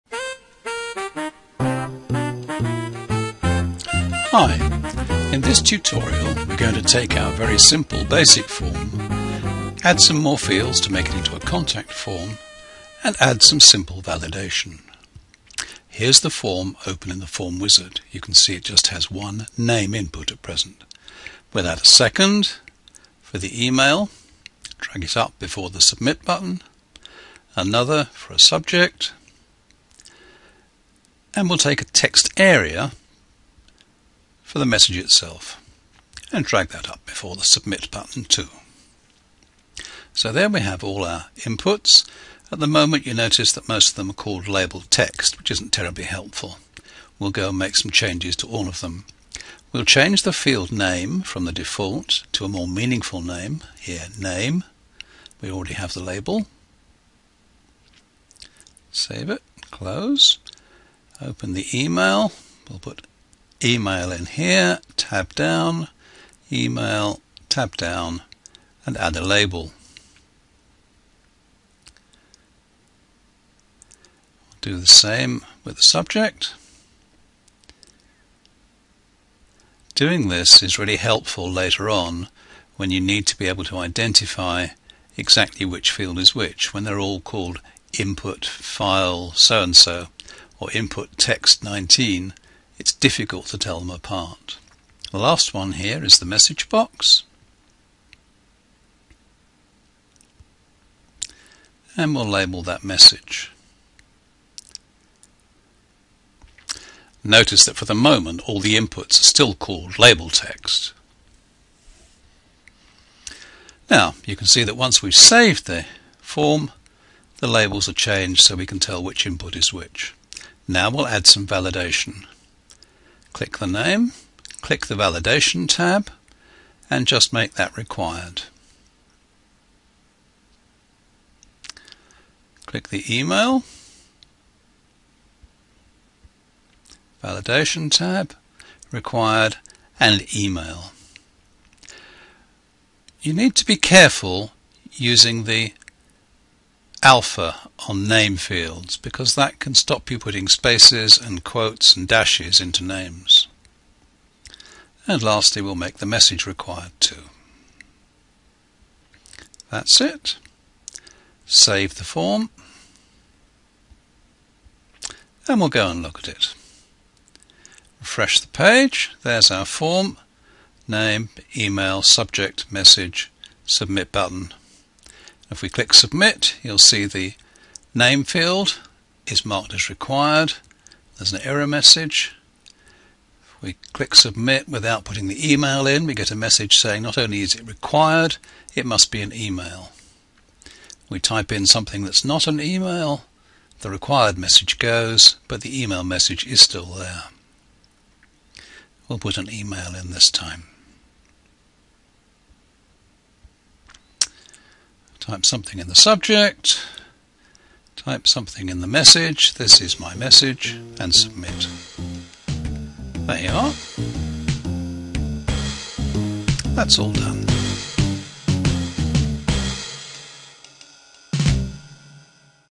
ChronoForms v4 : Validation
How to extend the form from the 'A first form' video into a contact form by adding more inputs - for 'email', 'subject' and 'message' - and some simple 'client-side' validation.